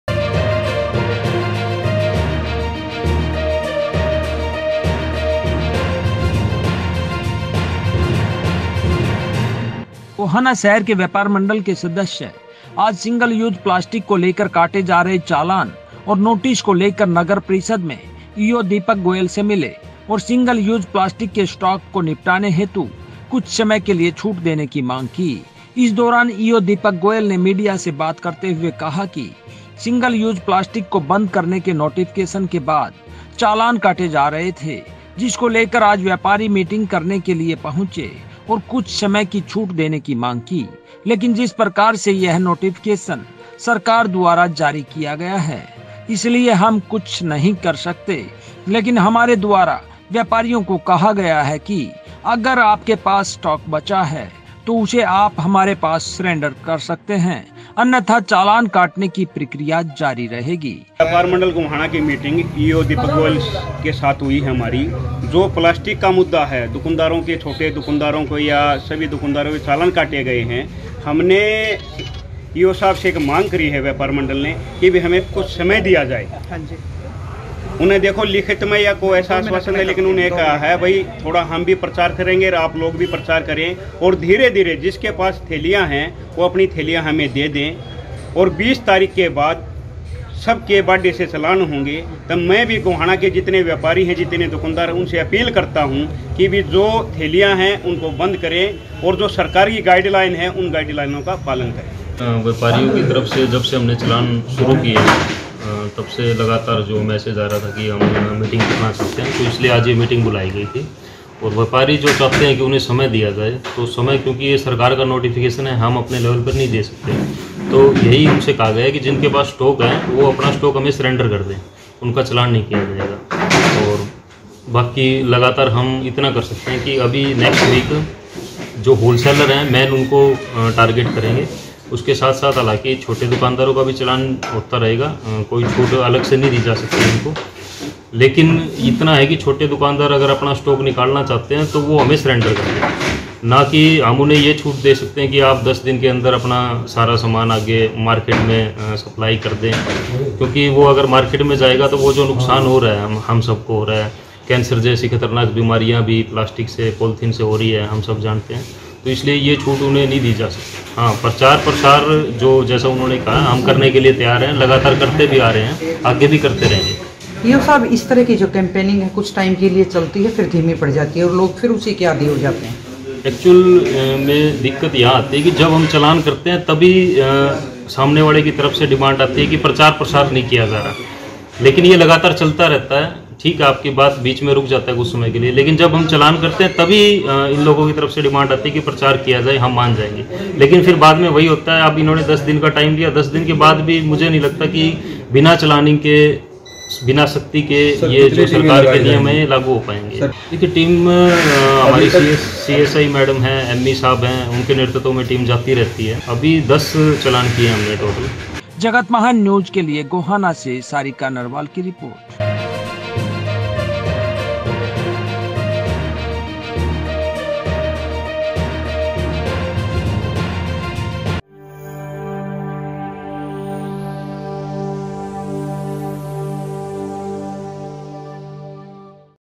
सिंगल यूज प्लास्टिक को लेकर काटे जा रहे चालान के लिए ईओ से क्यो मिले व्यापारी। देखिये एक रिपोर्ट
सिंगल यूज प्लास्टिक के स्टॉक को निपटाने हेतु कुछ समय के लिए छूट देने की मांग की
# व्यापार_मंडल #